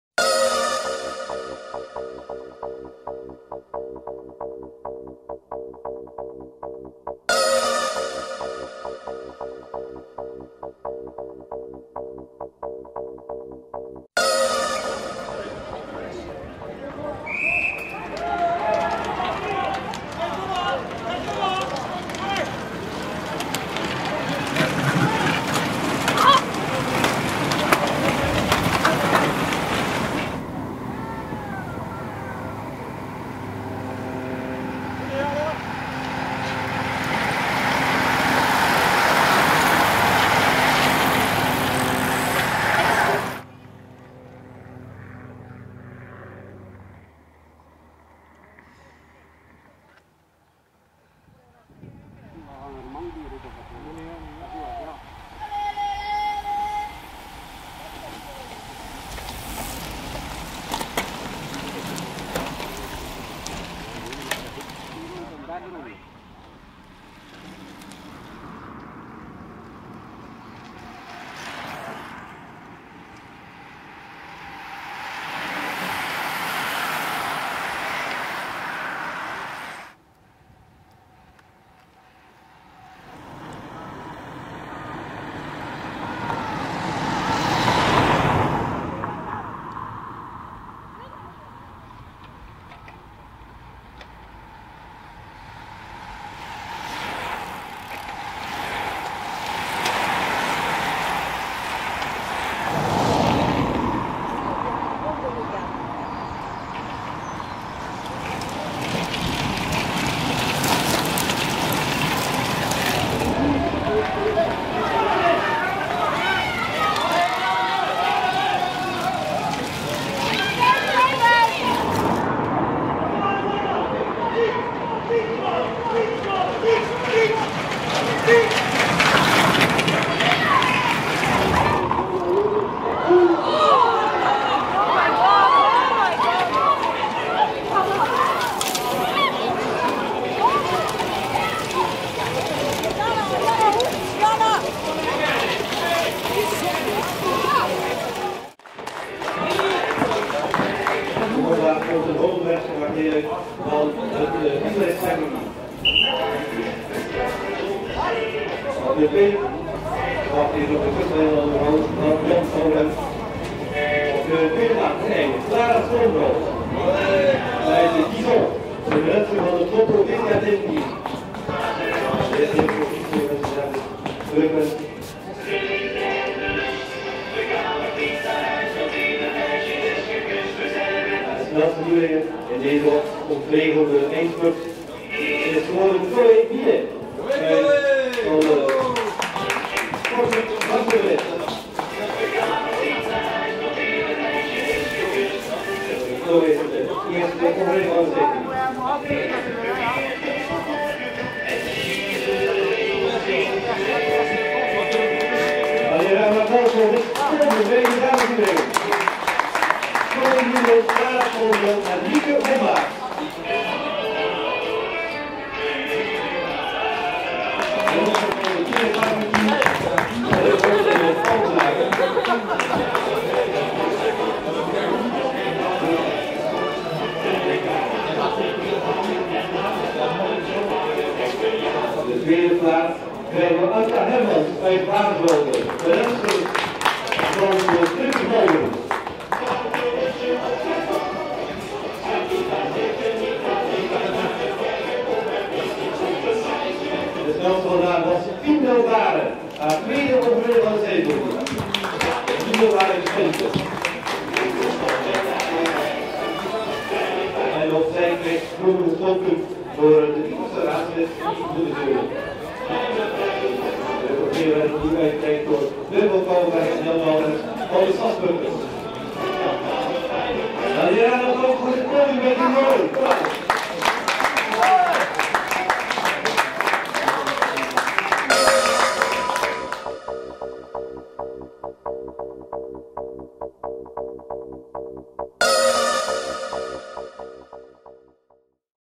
Boezinge 21/04/2013 Women Youth 1.16 N+J cycling video (race report)
Dames Nieuwelingen 1.16N      Boezinge  21/04/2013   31 Deelnemers   45.9 km   34.85  km/h

1 MINET Cloé  (BEL)              SPORTCLUB VELOCIP. - MARCHOVELETTE   01:25:12
2 SCHOONBROODT Clara (BEL)   LOTTO-VC ARDENNES                             z.t
3 MOMBAERTS Mieke  (BEL)         ISOREX CYCLING TEAM                         z.t

Dames Juniores 1.16J      Boezinge  21/04/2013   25 Deelnemers   45.9 km   34.85  km/h

1 DELBAERE Fien  (BEL)         YOUNG SCHELDE CT                                  01:25:12
2 HERMANS Anka  (BEL)         WIELERCLUB DE SPRINTERS MALDEREN     z.t
3 HELDERWEIRT Alicia (BEL)   ISOREX CYCLING TEAM                                 z.t